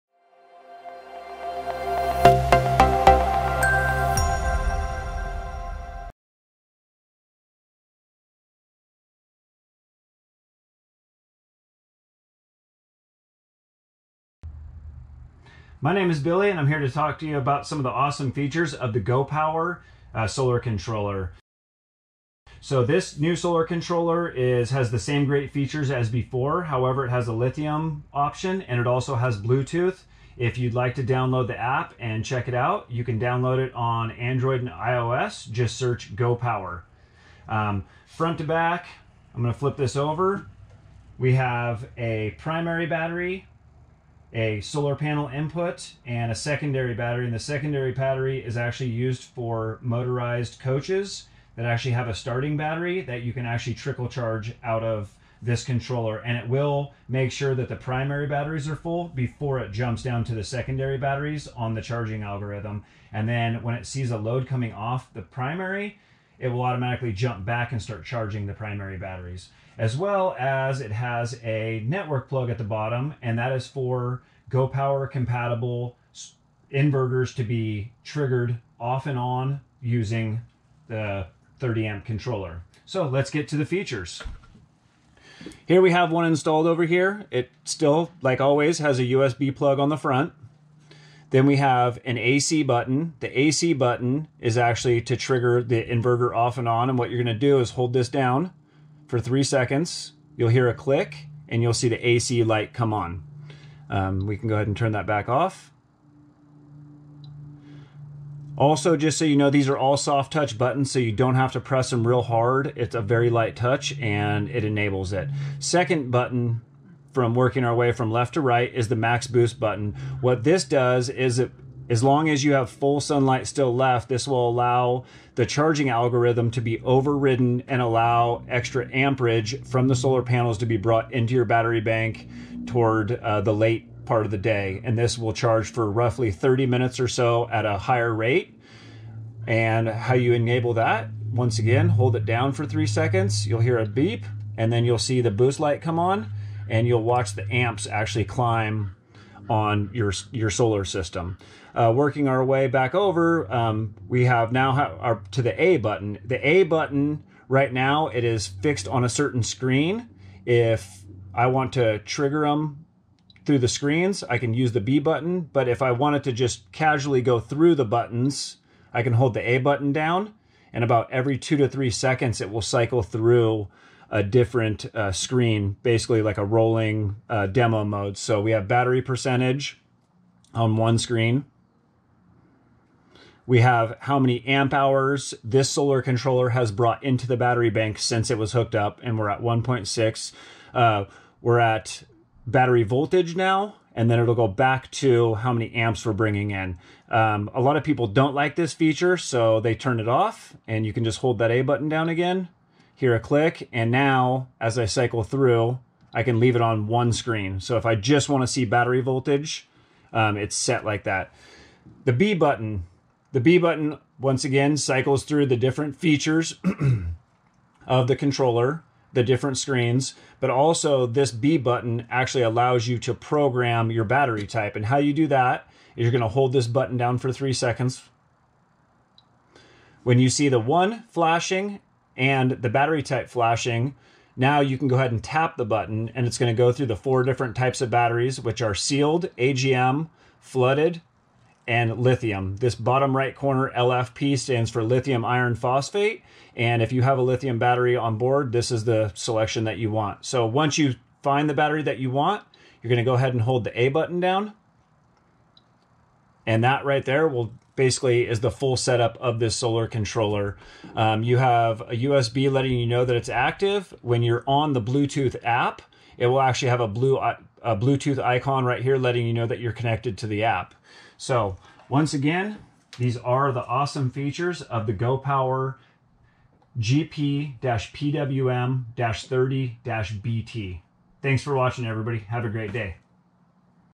Go Power! Bluetooth Solar Controller 101
Join Billy Slade from System Solution to discover all the great features of the Go Power! Bluetooth Solar Controller.

Find out more about our many complete large or small solar systems and accessories online at gopowersolar.com